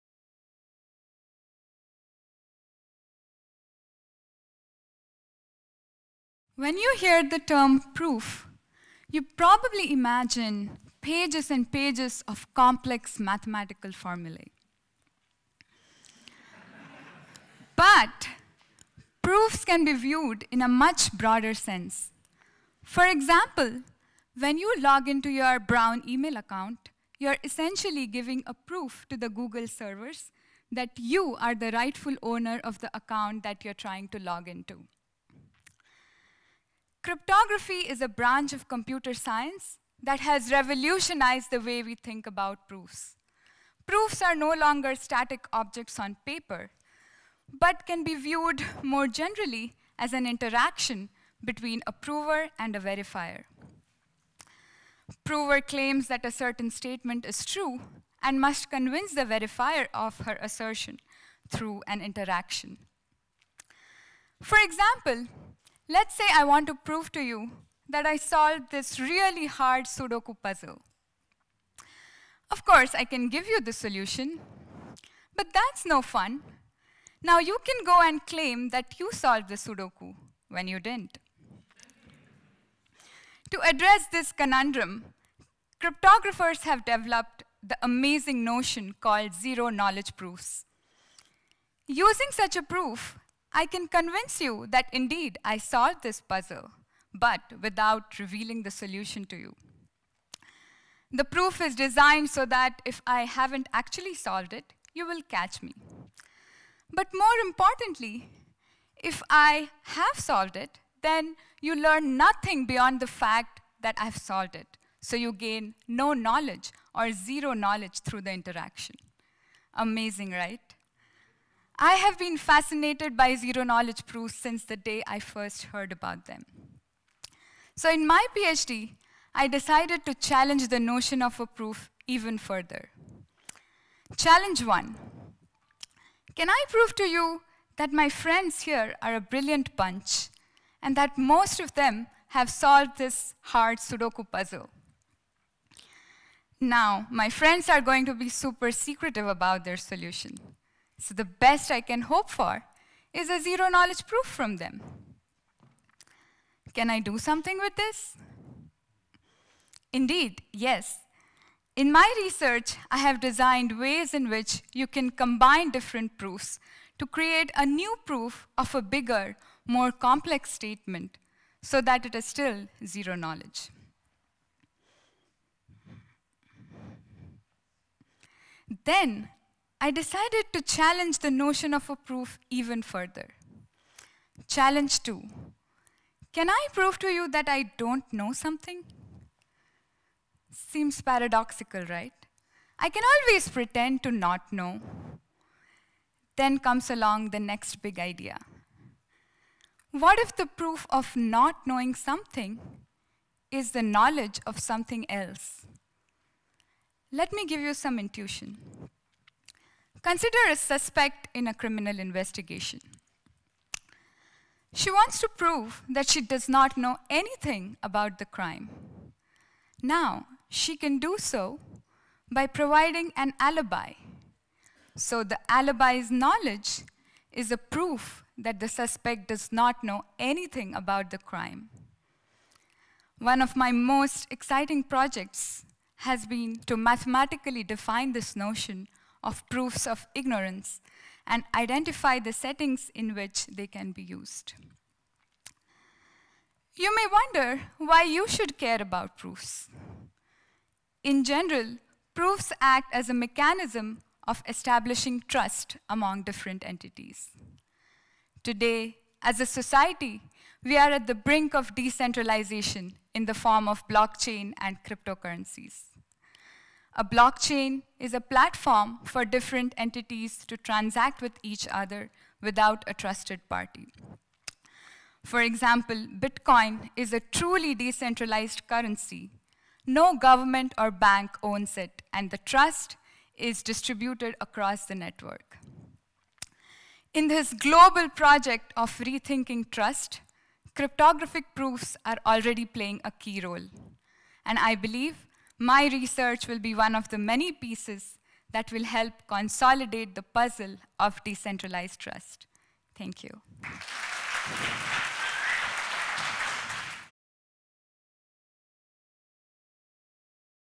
Apoorvaa Deshpande: Decentralizing Trust through Cryptographic Proofs
Apoorvaa Deshpande, PhD Candidate in Computer Science, describes her research on constructing extensions to zero-knowledge proofs for the purpose of creating privacy-preserving tools for the blockchain.

This talk was part of Research Matters, featuring short talks about research by Brown University Graduate Students on April 9, 2019.

Brown University